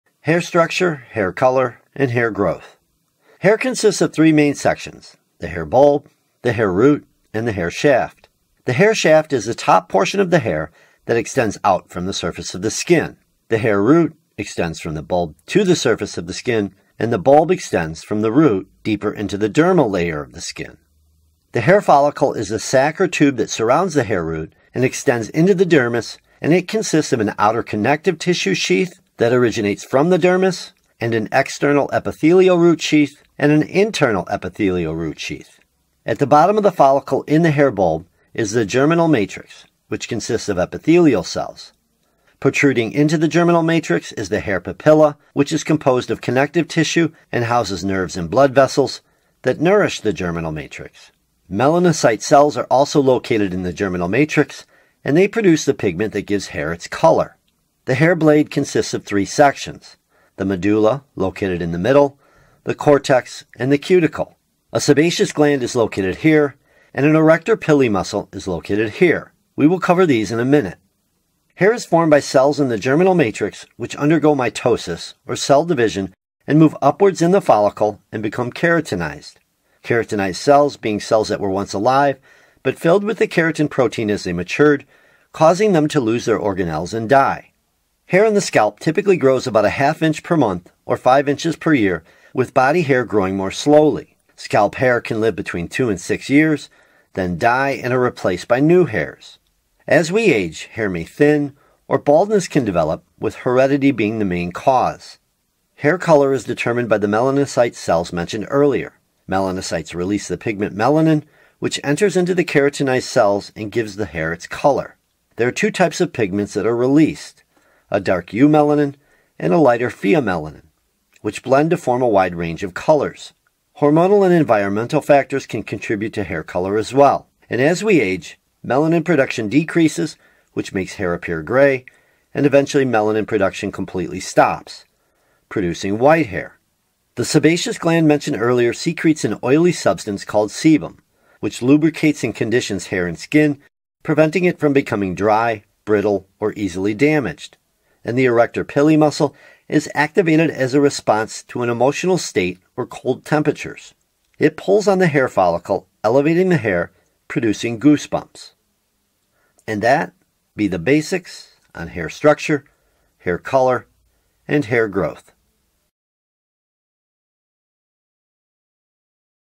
Structure Of Hair Follicle - Hair Color - How Does Hair Growth Work
In this video we discuss the structure of hair, how does hair grow and how does hair color work?  We also look at the function of the sebaceous gland and the function of the arrector pili muscle.

Transcript/notes

Hair structure, hair color and hair growth.

Hair consists of 3 main sections, the hair bulb, the hair root and the hair shaft.  The hair shaft is the top portion of the hair that extends out from the surface of the skin.  The hair root extends from the bulb to the surface of the skin, and the bulb extends from the root deeper into the dermal layer of the skin.

The hair follicle is the sac or tube that surrounds the hair root and extends into the dermis and it consists of an outer connective tissue sheath that originates from the dermis and an external epithelial root sheath and an internal epithelial root sheath.

At the bottom of the follicle in the hair bulb is the germinal matrix which consists of epithelial cells.  Protruding into the germinal matrix is the hair papilla which is composed of connective tissue and houses nerves and blood vessels that nourish the germinal matrix.  Melanocyte cells are located in the germinal matrix, and they produce the pigment that gives hair its color.

The hair blade consists of 3 sections, the medulla, located in the middle, the cortex and the cuticle.

A sebaceous gland is located here, and an arrector pili muscle is located here.  We will cover these in a minute.

Hair is formed by cells in the germinal matrix which undergo mitosis, or cell division, and move upwards in the follicle and become keratinized.  Keratinized cells being cells that were once alive, but filled with the keratin protein as they matured, causing them to lose their organelles and die.

Hair on the scalp typically grows about a ½ inch per month or 5 inches per year with body hair growing more slowly.  Scalp hair can live between 2 and 6 years, then die and are replaced by new hairs.  As we age hair may thin, or baldness can develop with heredity being the main cause.

Hair color is determined by the melanocyte cells mentioned earlier.  Melanocytes release the pigment melanin which enters into the keratinized cells and gives the hair its color.  There are 2 types of pigments that are released, a dark eumelanin and a lighter pheomelanin, which blend to form a wide range of colors.  Hormonal and environmental factors can contribute to hair color as well.  As we age, melanin production decreases which makes hair appear grey and eventually melanin production completely stops producing white hair.

The sebaceous gland mentioned earlier secretes an oily substance called sebum which lubricates and conditions hair and skin preventing it from becoming dry, brittle or easily damaged.  And the arrector pili muscle is activated as a response to an emotional state or cold temperatures.  It pulls the hair follicle, elevating the hair, producing goose bumps.

Timestamps
0:00 The 3 main sections of hair
0:21 The hair follicle
1:11 How hair is formed
1:27 Hair growth over time
1:45 Hair color